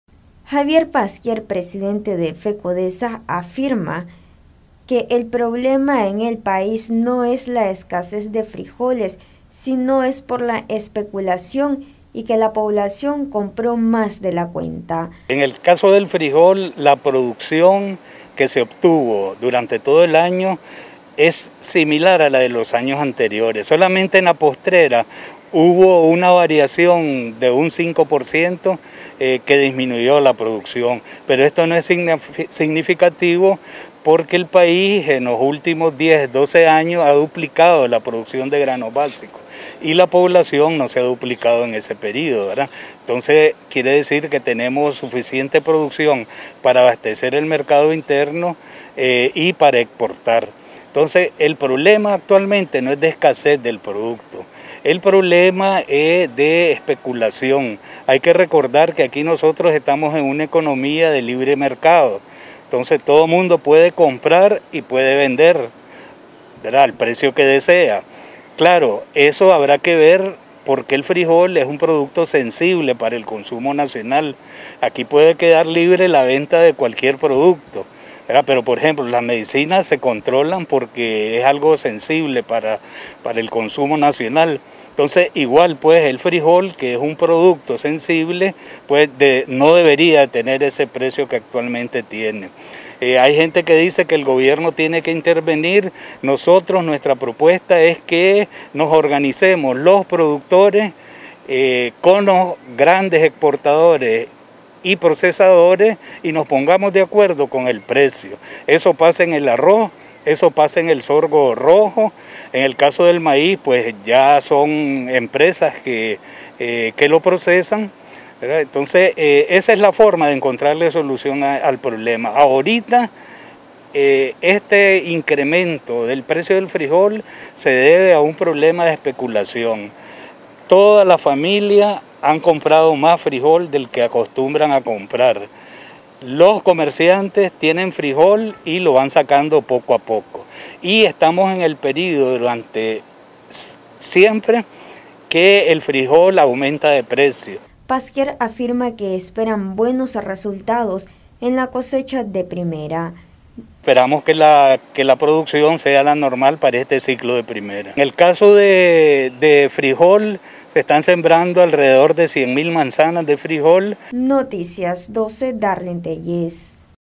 “Hay suficiente producción de frijoles para abastecer los mercados”
Alza de la libra de frijoles se debe a la especulación y porque la población compró más de la cuenta, afirma Javier Pasquier presidente de La Federación de Cooperativas para el Desarrollo